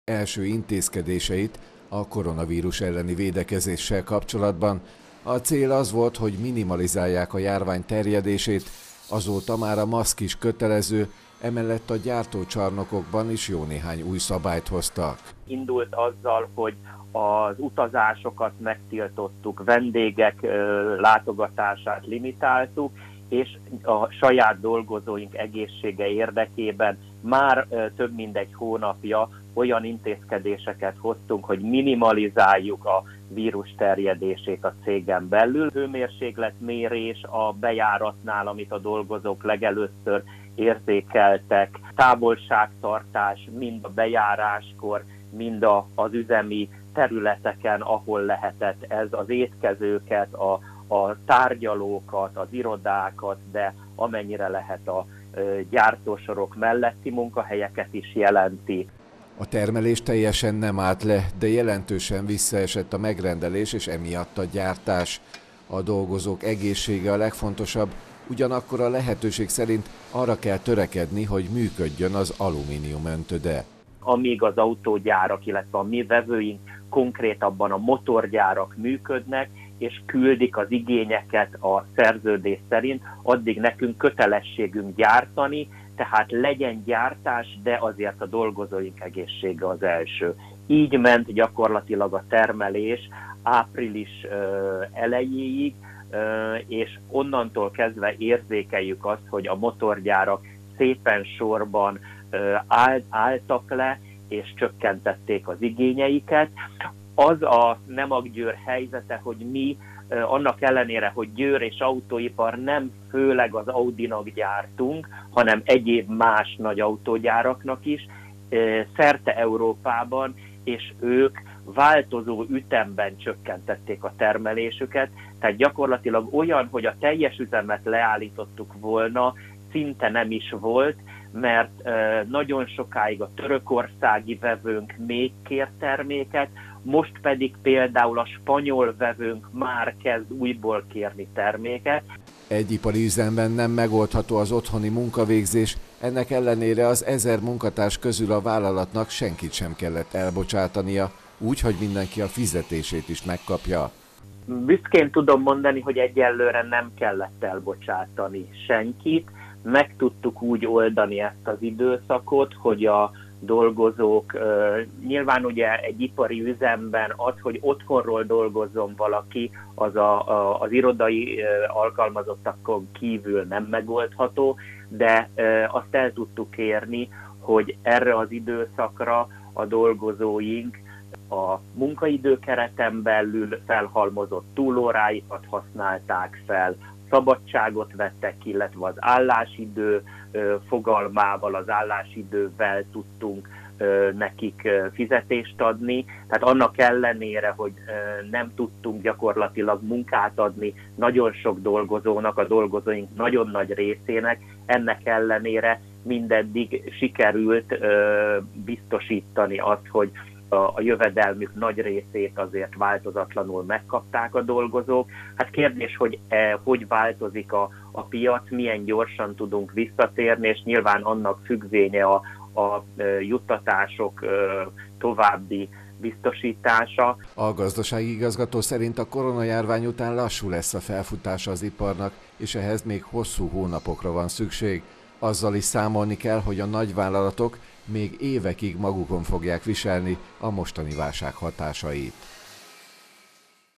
Lassú felfutásra számít a Nemak
Bár jelentősen csökkentette termelési kapacitását a Nemak Győr, az alumínium öntöde nem állította le üzemeit.  Az ezer munkatárs közül a vállalatnak senkit sem kellett elbocsátania, úgy hogy mindenki a fizetését is megkapja. Mersich Gábor gazdasági igazgató szerint a koronajárvány után lassú lesz a felfutása az iparnak, de ehhez még hosszú hónapokra van szükség. Azzal is számolni kell, hogy nagyvállalatok még évekig magunkon fogják viselni a mostan válság hatásait.